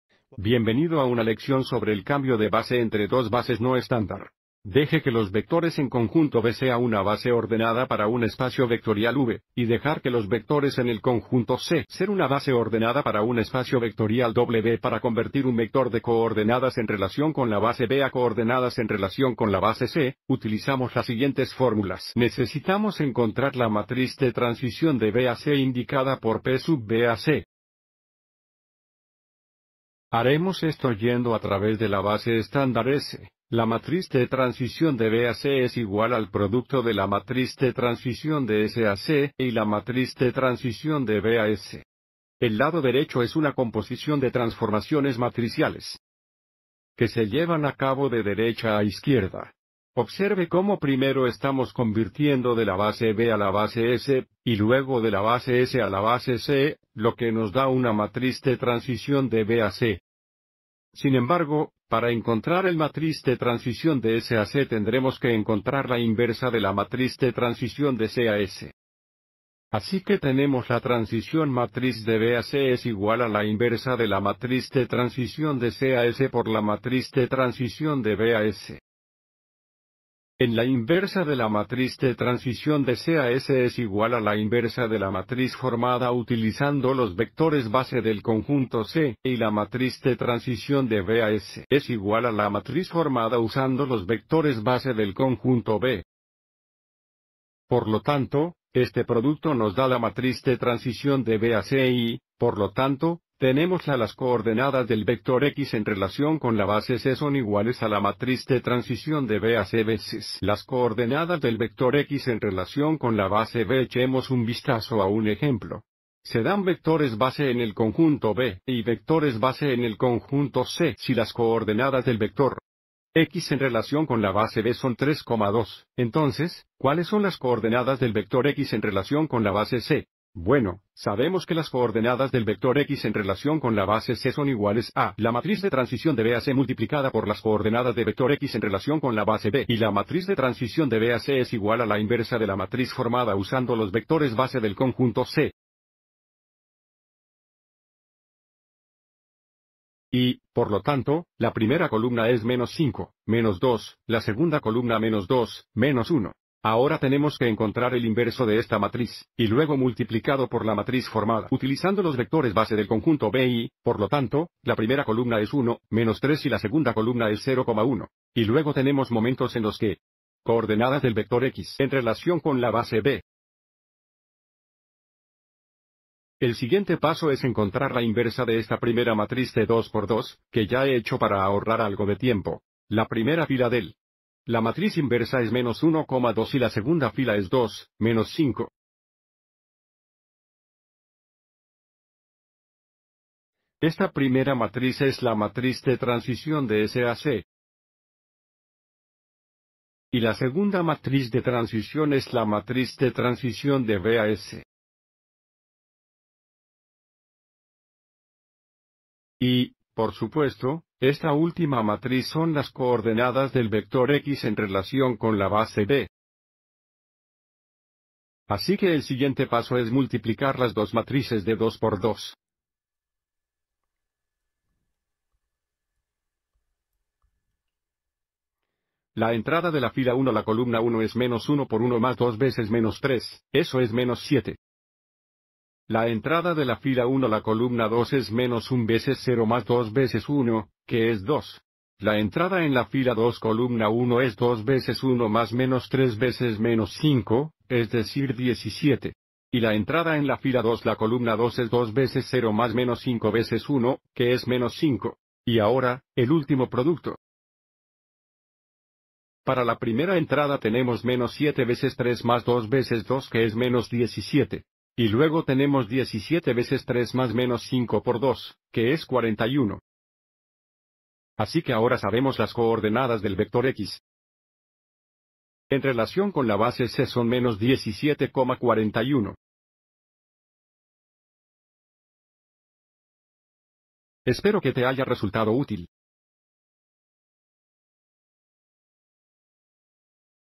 Introducción al cambio de base entre dos bases no estándar
Este vídeo presenta un cambio de base y muestra cómo convertir entre dos bases no estándar.